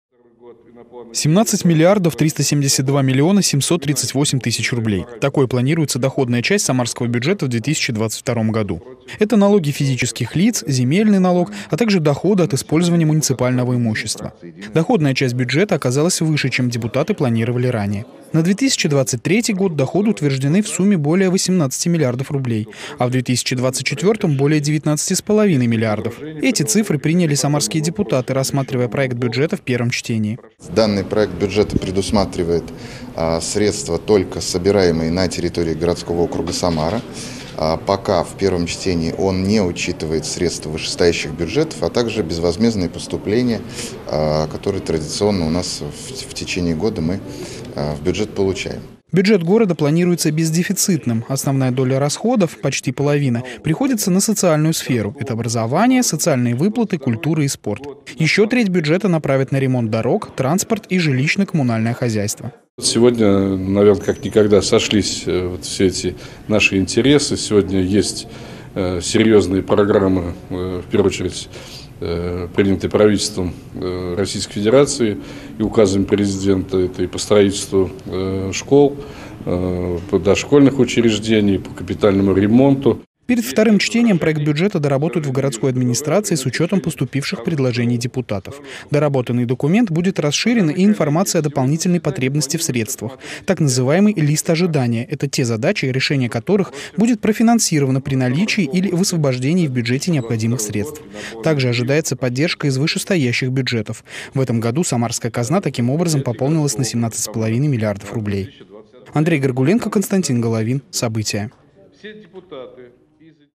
Депутаты гордумы приняли проект бюджета Самары на 2022 год в первом чтении
Депутаты самарской городской Думы в первом чтении приняли проект бюджета Самары на 22 год и на плановый период 23-го и 24-го годов. Планируется, что казна будет бездефицитной. И доходы, и расходы утверждены в сумме более 17 миллиардов рублей. Это без учета поступлений из вышестоящих бюджетов. Расходы сохранят социальную направленность. Подробнее в следующем сюжете.